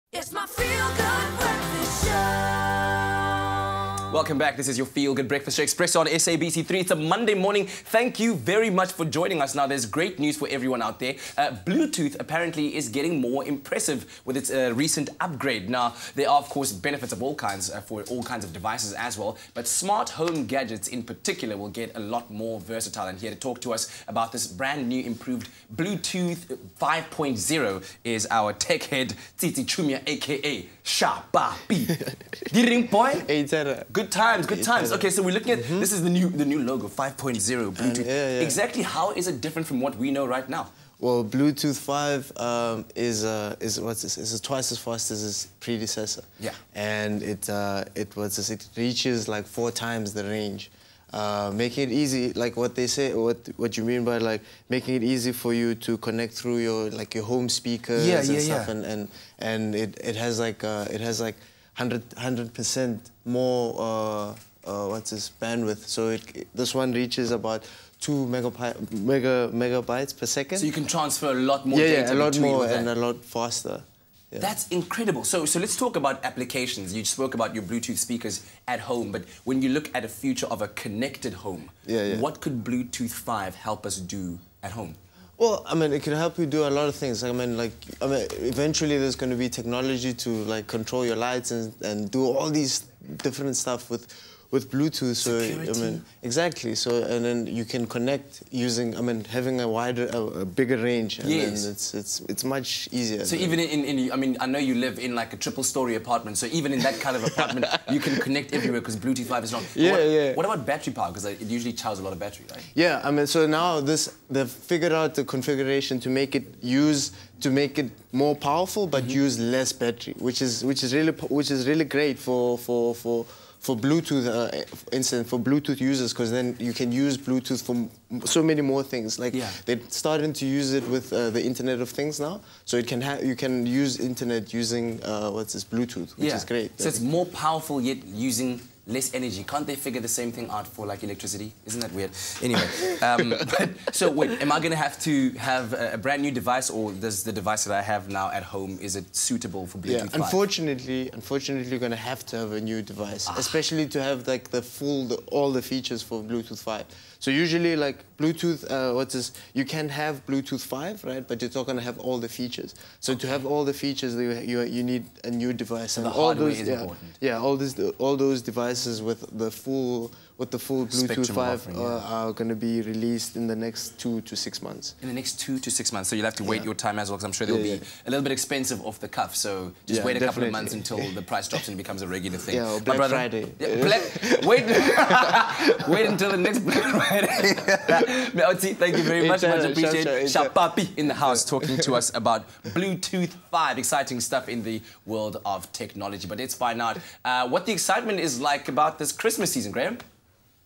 TECH DAILY - New Bluetooth 5 is super charged!
Great news for everyone: Bluetooth just got an impressive upgrade. There are benefits for all kinds of devices, but smart home gadgets in particular will get a lot more versatile. Here to talking about the new and improved Bluetooth 5.0 is tech head and game designer Tsitsi Chiumya